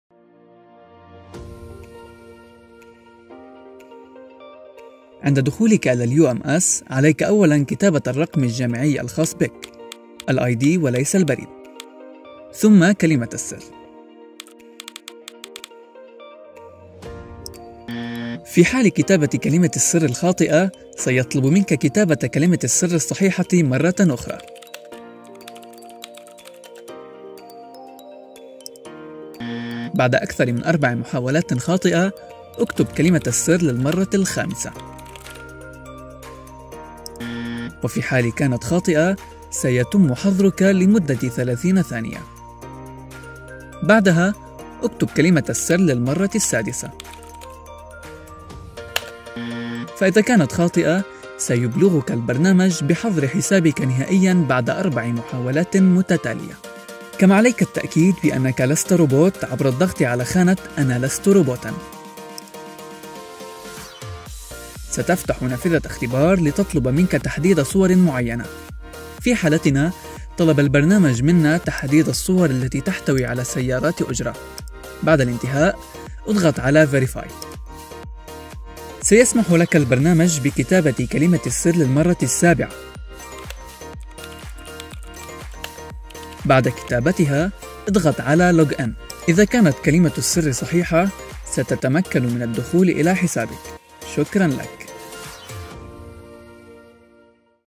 UMS Tutorials: Entering Wrong Password | ادخال كلمة مرور خاطئة
UMS Tutorials: What should you do when you enter the wrong password?

ماذا تفعل في حال ادخلت كلمة مرور خاطئة

MU​​, Mastering Life with Wisdom & Knowledge.